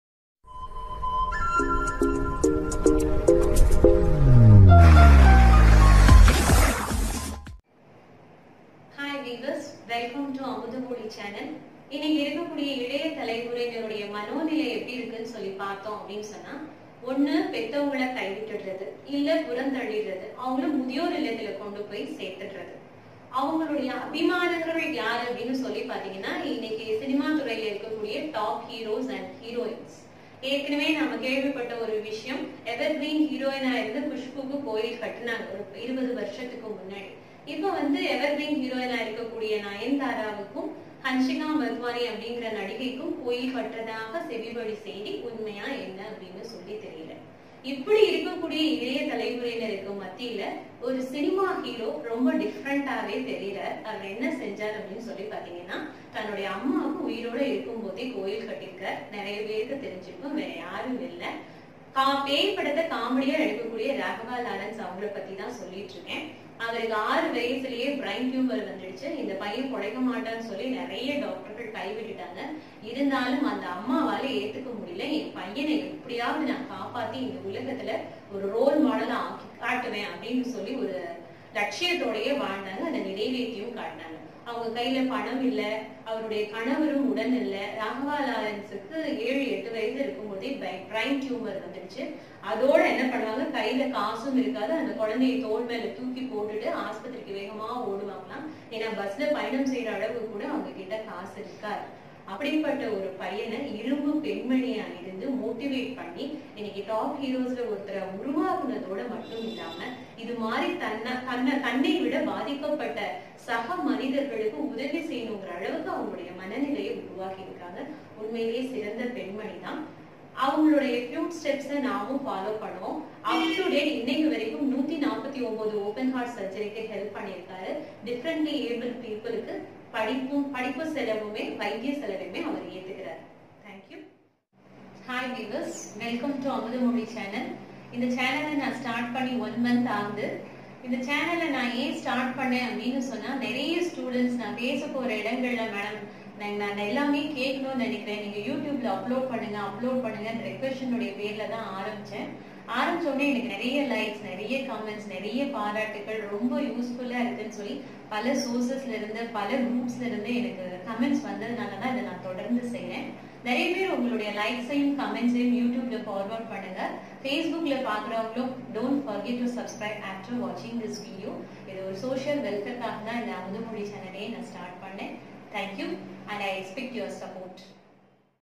/Amuthamozhi/இளைஞர்களுக்கு ஓர் வேண்டுகோள்/request to the youngsters
Dr.M.kiruthika somasuntharam.PhD.,
Motivational & Spiritual orator.
Assistant professor in English
 இளைஞர்கள் பெற்றவர்களுக்கு கோயில் கட்டி வணங்கவில்லை என்றால் பரவாயில்லை  , அவர்களை முதியோர் இல்லங்களில் கொண்டு போய் விட வேண்டாம். அவர்களின் இதய கோவில்களில் சிம்மாசனம் போட்டு அமர்ந்து இருப்பவர்கள் ,அவர்களின் தவபுதல்வர்கள் நீங்கள் மட்டுமே.